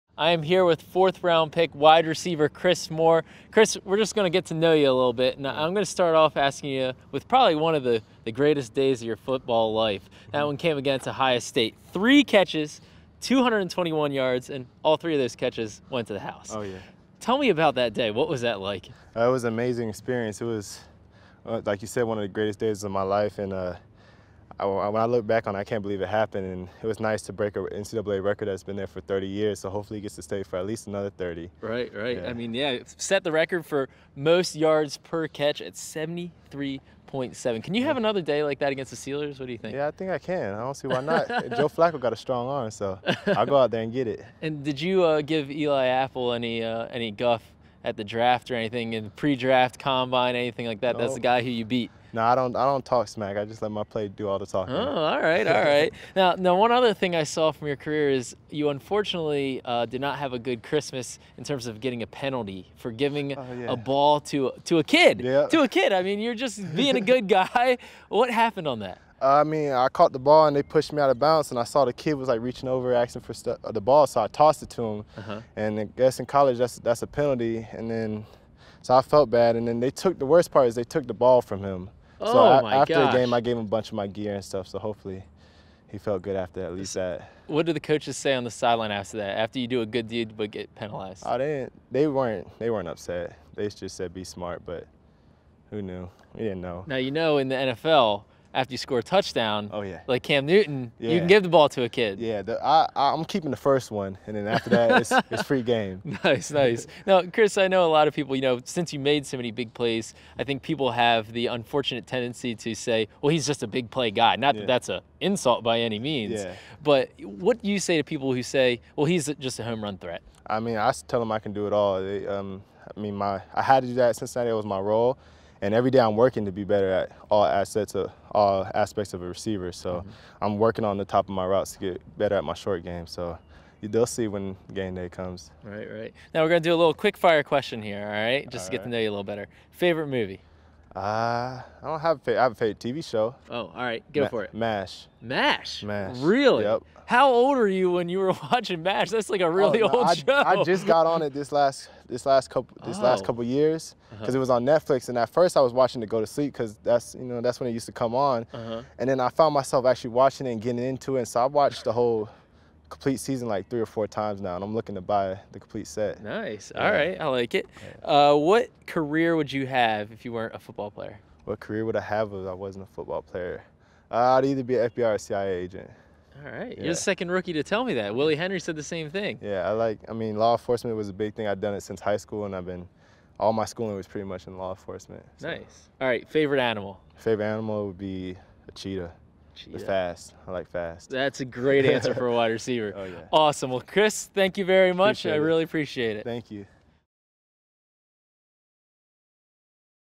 WR Chris Moore | Meet The Rookies | Baltimore Ravens
The greatest day of the rookie’s life was an insane game in which he set the NCAA record for most yards per catch. Plus, Moore revealed his favorite show is M*A*S*H, which ran on TV a decade before he was even born.